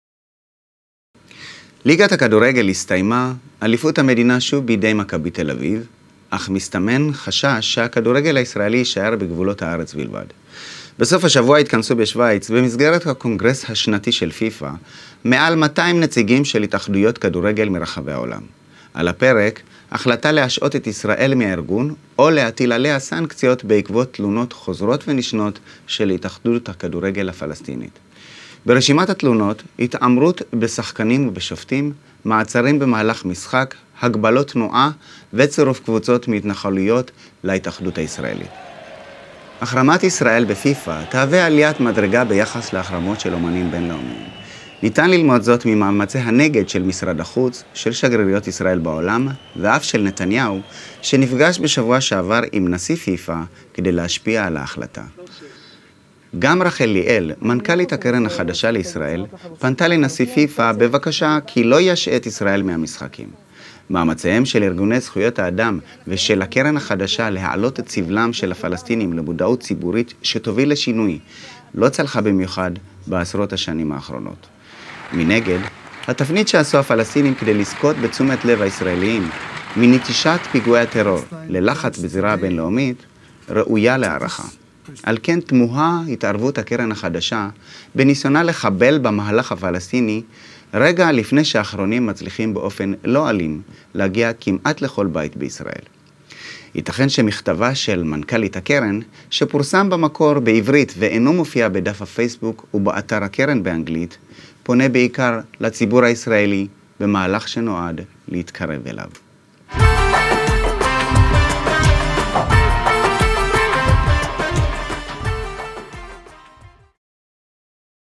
Own Goal
Switzerland will gather this weekend, as part of FIFA's annual congress, over 200 representatives of the football associations around the world. On the agenda, Israel's decision to suspend the organization, or to impose sanctions following the repeated complaints of the Palestinian Football Association. FIFA boycott of Israel would be a step in relation to boycotts of international artists. Also executive director of the New Israel Fund, FIFA president appealed, asking that Israel did not heed the games. The efforts of human rights organizations and the new fund to raise the plight of the Palestinian public awareness would lead to a change, did not work particularly in recent decades. On the other hand, the turnaround made by the Palestinians to get attention Israelis - abandonment of terrorist attacks, international pressure, is admirable. Therefore surprising new fund intervention during its attempt to sabotage Palestinian, just before the latter, successful non-violent struggle to reach nearly every home in Israel. Perhaps the Foundation director general's letter, originally published in Hebrew, and does not appear on the Facebook page and on English horn, primarily addressing the Israeli public, a move intended to approach him.